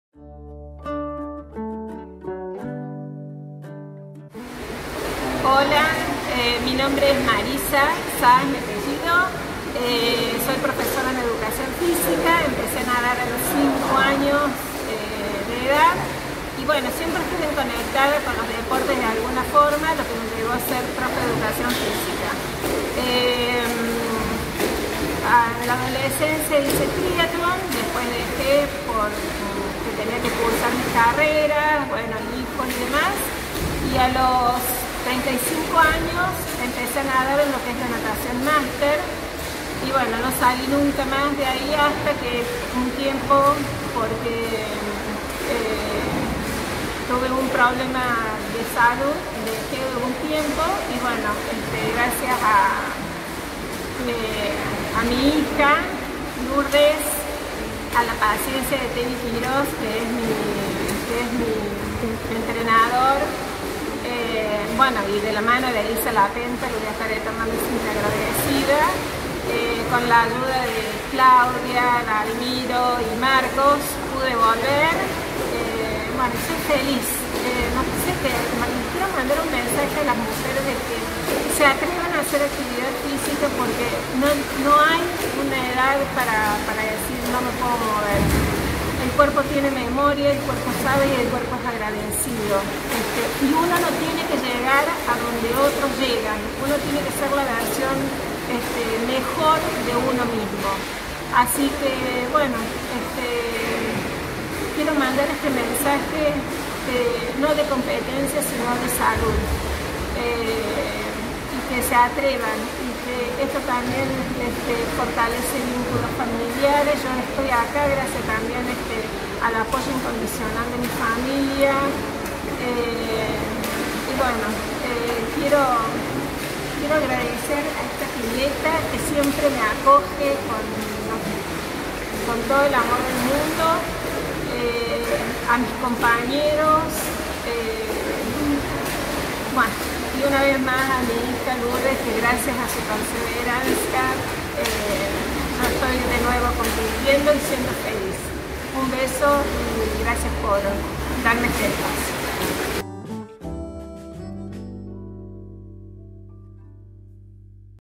Marisa Sá en YoPuedo
Un ciclo especial de la Confederación de Deportes que se desarrollará  en el marco de la Comisión Mujer en el Deporte de la Confederación. Tiene como objetivo promover la participación e inclusión más mujeres en cargos de conducción y gestión institucional.